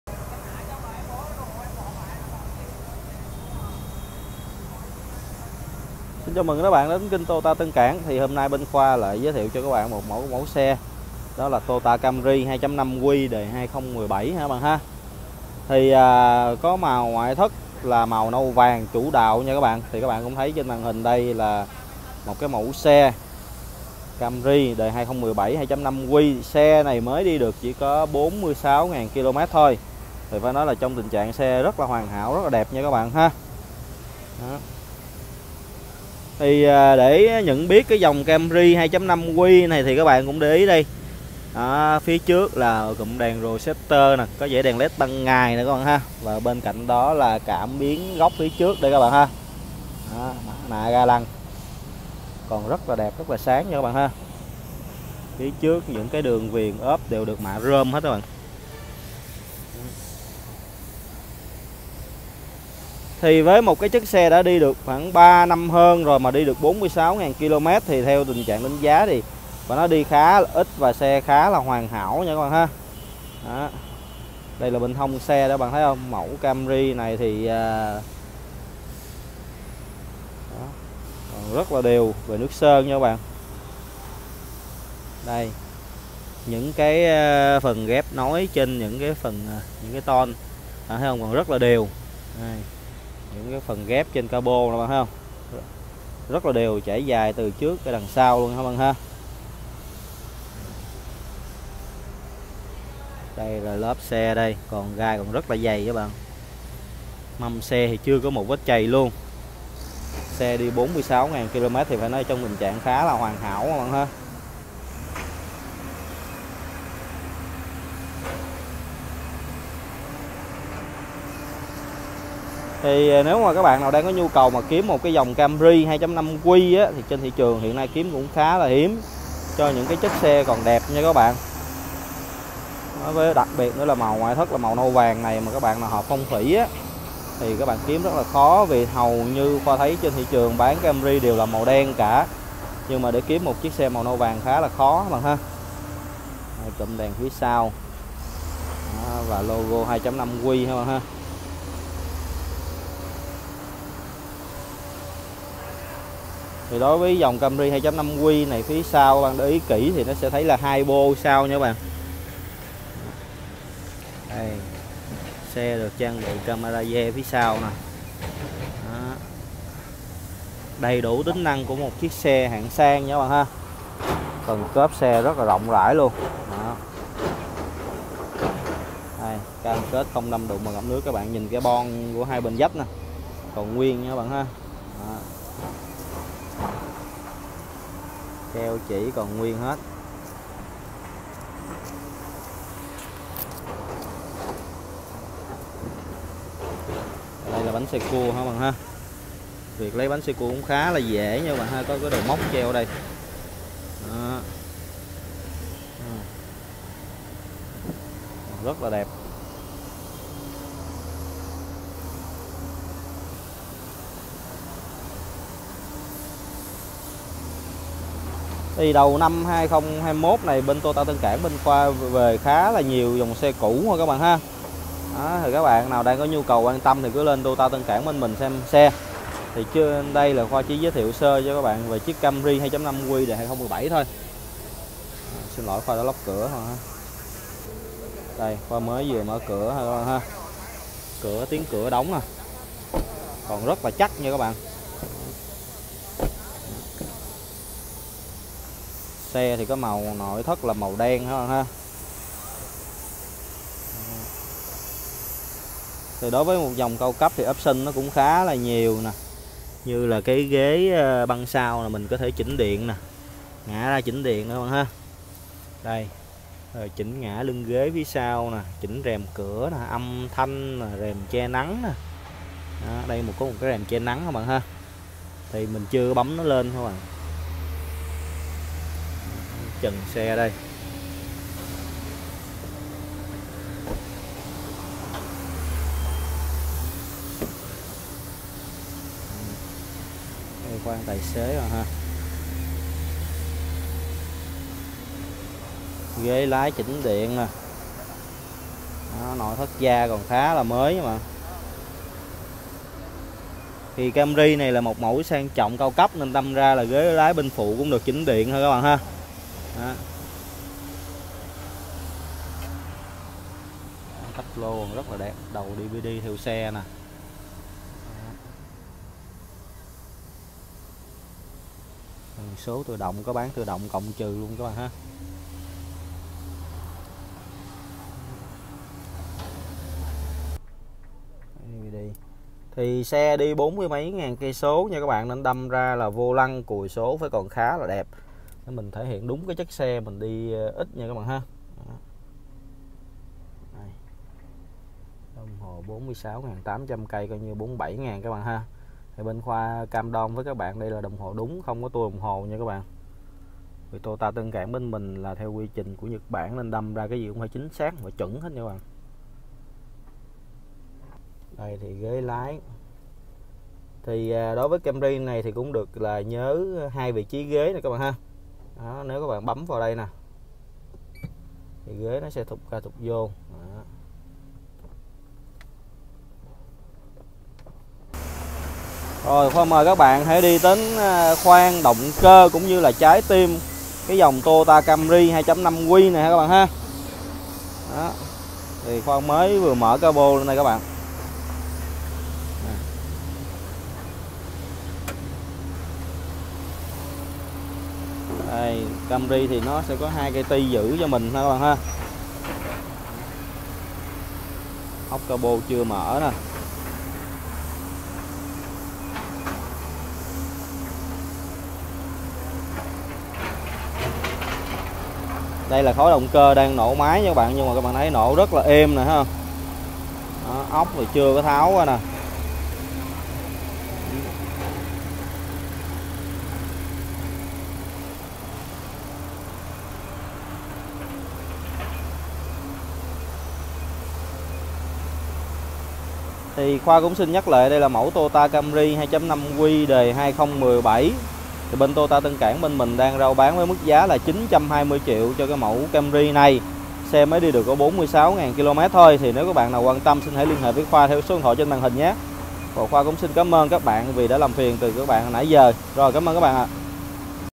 Toyota Tân Cảng cần bán Camry 2.5Q đời 2017 xe đẹp đi ít - Mua xe ô tô cũ các tỉnh giá tốt
Bán Toyota Camry 2.5Q đời 2017
Xe đi được 46.900km trang bị đầy đủ OPtion
Đã được Toyota Tân Cảng kiểm tra 176 hạng mục
Đảm bảo xe không đâm đụng và ngập nước
Hỗ trợ Vay Ngân Hàng lên đến 70% giá trị xe
Chương trình đổi xe cũ với giá cực tốt